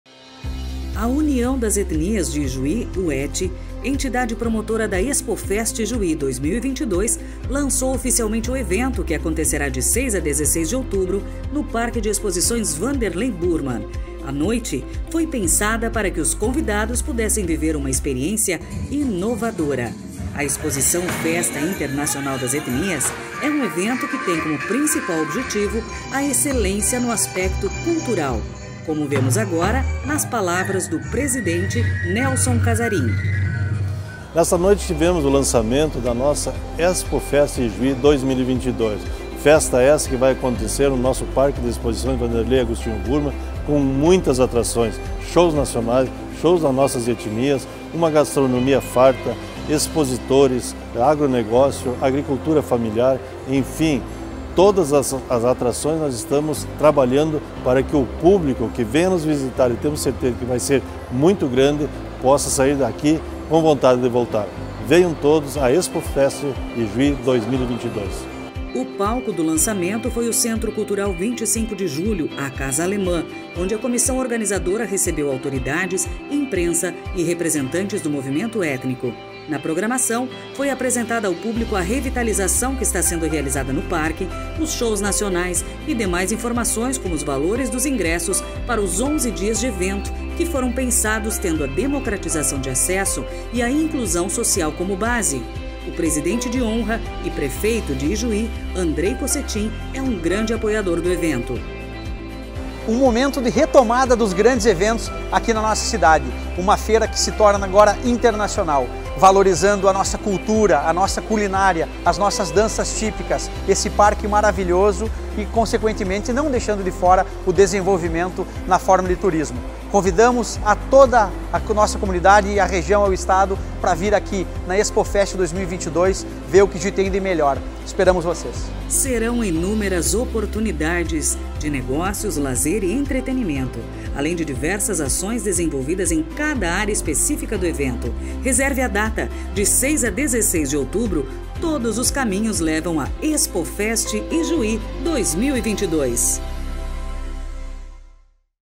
Lançamento Oficial ExpoFest Ijuí 2022 | Informativo ExpoFest
E assim foi a noite do meu lançamento!

Em outubro a Capital Nacional das Etnias te espera para o maior evento do ano!

🚀 Expectativa de público superior a 200 mil pessoas!
🚀 11 dias de evento (de 6 a 16 de outubro)!
🚀 Mais de 480 espaços de exposição (cerca de 60% são expositores de Ijuí)
🚀 Mais de 200 shows culturais durante os 11 dias de festa!
🚀 Shows Nacionais (Hugo & Guilherme / Jorge e Mateus)!
🚀 E muito, muito mais!

Já sabe né?👀 De 6 a 16 de outubro, todos os caminhos levam à ExpoFest Ijuí 2022 no Parque de Exposições Wanderley Burmann.

Somos Todos, Somos Um.

🔜Agora a Expo é Fest! Em outubro a CapitalNacionalDasEtnias te espera.😉 Acompanhe todas as novidades no